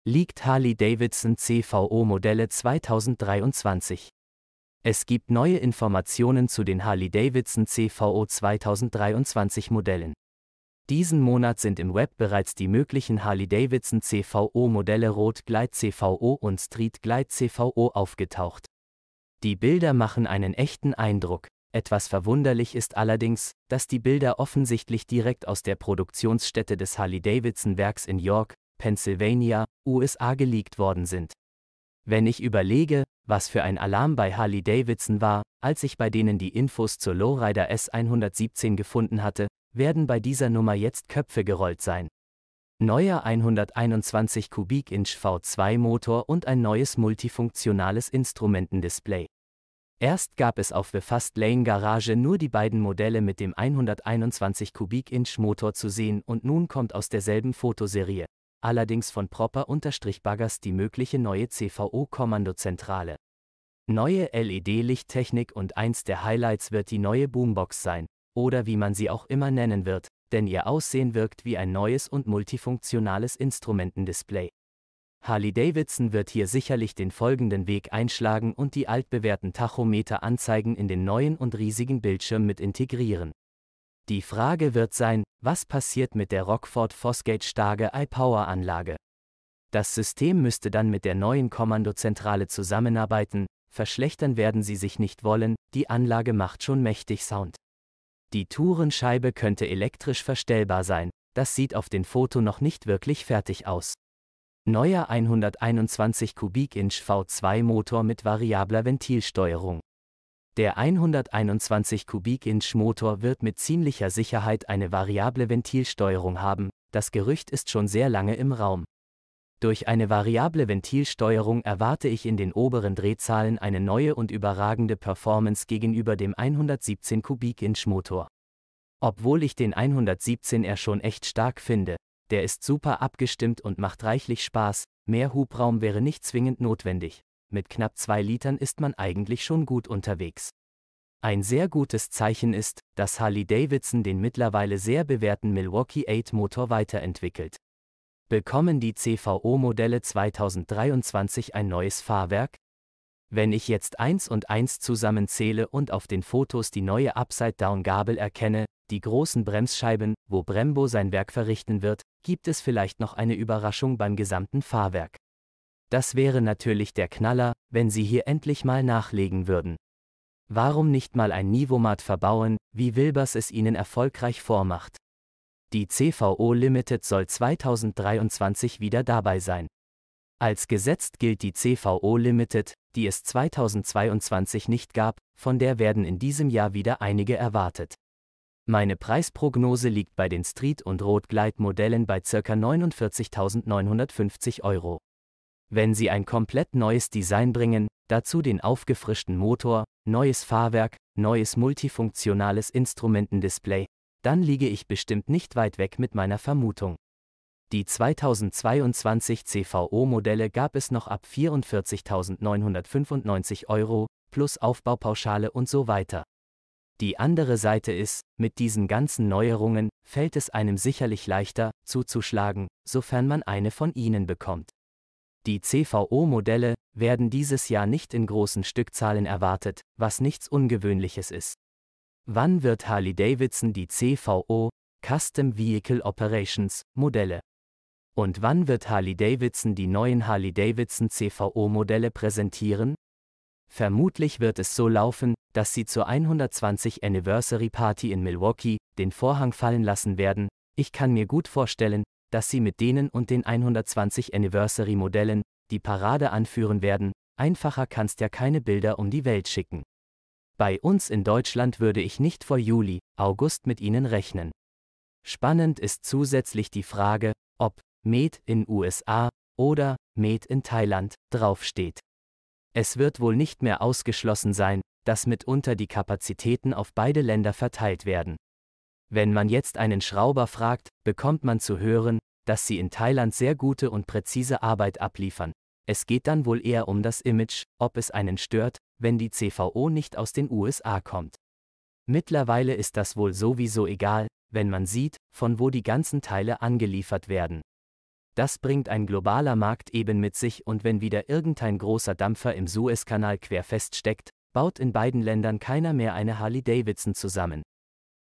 LEAKED - HARLEY-DAVIDSON CVO MODELLE 2023 (AI)
Es gibt neue Informationen zu den Harley-Davidson CVO 2023 Modellen
Diesen Monat sind im Web bereits die möglichen Harley-Davidson CVO Modelle Road Glide CVO und Street Glide CVO aufgetaucht. Die Bilder machen einen echten Eindruck, etwas verwunderlich ist allerdings, das die Bilder offensichtlich direkt aus der Produktionsstätte des Harley-Davidson Werks in York, Pennsylvania, USA geleakt worden sind.